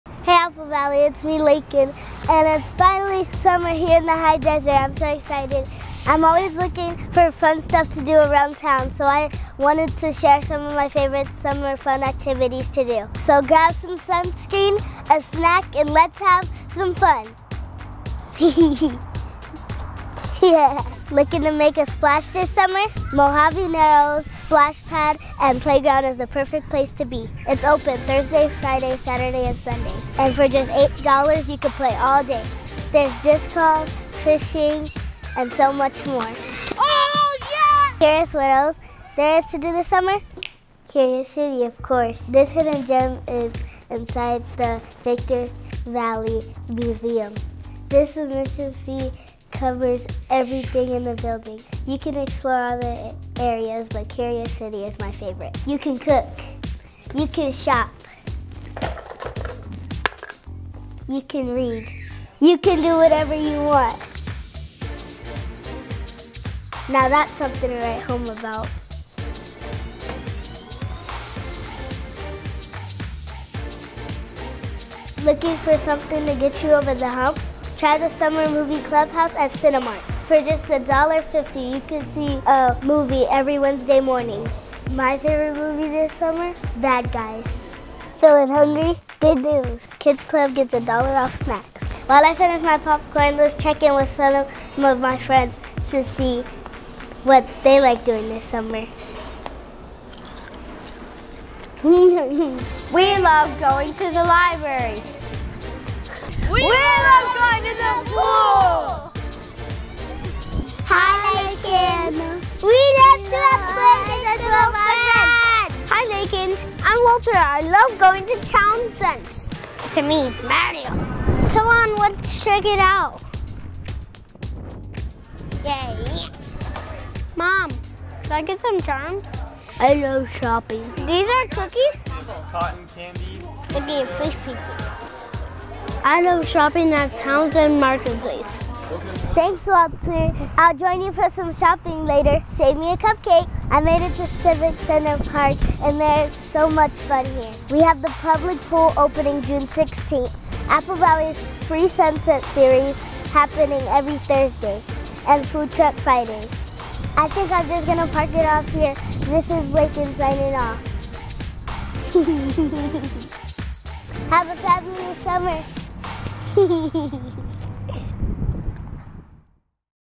Fun Things To Do Around Apple Valley this Summer
Leikyn shows us all her favorite summer spots for kids around Apple Valley!